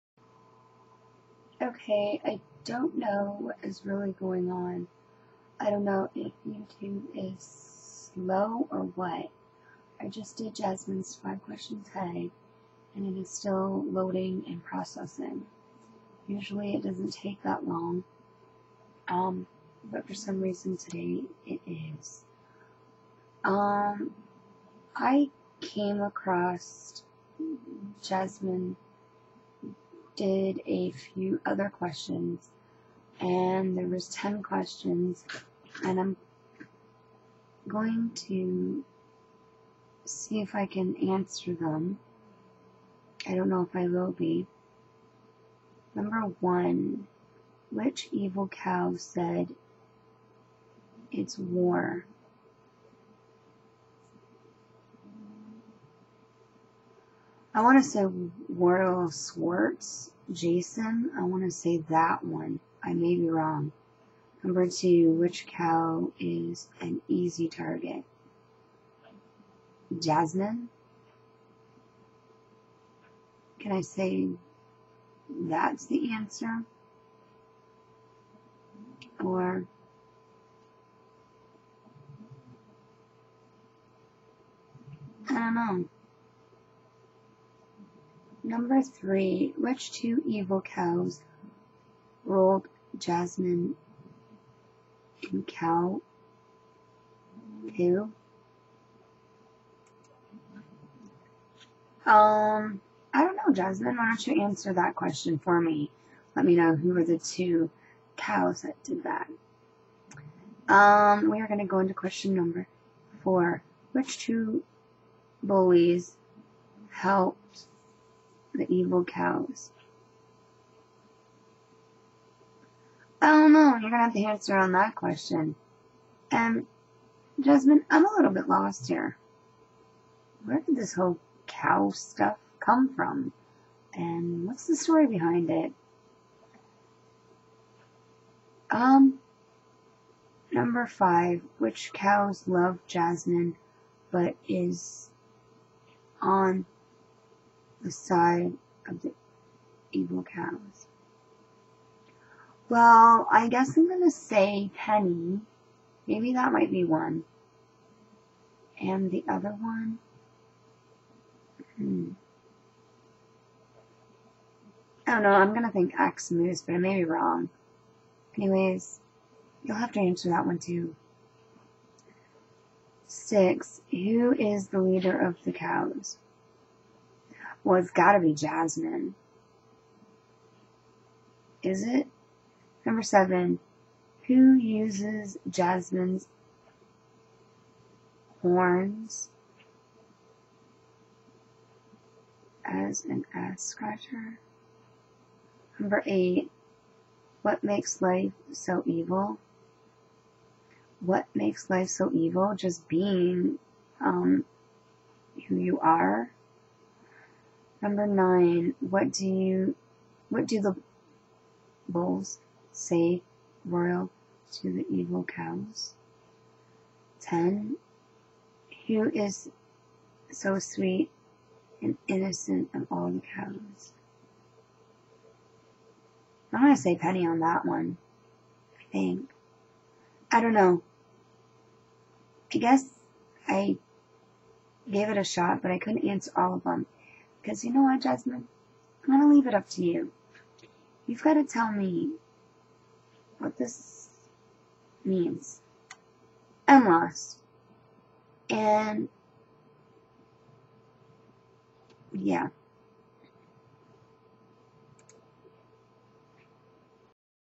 10 questions
1. which evil cow said. ( its war )
2. which cow is an easy target
3. which 2 evil cows rolled Jasmine in cow poo
4. which 2 bull's helps the evil cows
5. which cow loves Jasmine but is on the side of the evil cows
6. who is the leader evil cow
7. who uses Jasmine's horns as a ass scratcher
8. what makes Life so evil
9. why do the bulls stay royal to the evil cows
10. who is so sweet and innocent of all cows